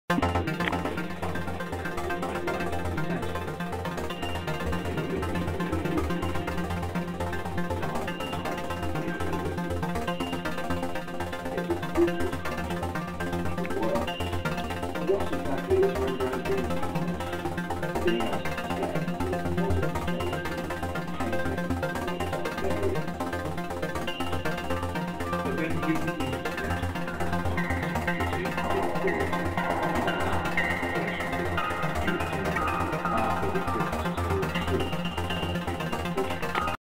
w pracowni e`ryka malarza - Poland - luty 2012
EL muzyka - eryk malarz, malarstwo - e`ryk - czyli - w pracowni Eryka Malera - muzy i kolekcjonerów malarstwa z energią ha zapraszam do kontaktów
ERYK MALER - MALARSTWO SZTALUGOWE I MUZYKA - malarstwo dźwiękowe do filmów i reklam. EL music - Erik painter, painting - e `roar - that is - in the studio of Eric Maler - muse art and art collectors with energy ha invite you to contact -
Eryk Maler - easel painting and music - painting sound for movies and commercials.